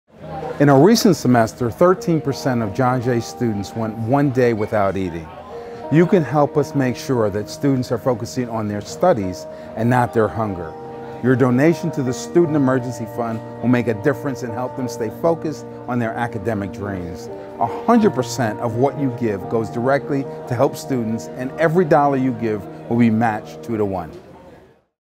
It’s Hard to Study When You’re Hungry
Annual Day of Giving is a John Jay College community-wide effort to raise money for the Student Emergency Fund which provides relief to students facing homelessness, food insecurity, medical emergencies, family crises and other difficulties so they can stay in school.

Every $1 donated will be matched by $2 from the Carroll and Milton Petrie Foundation.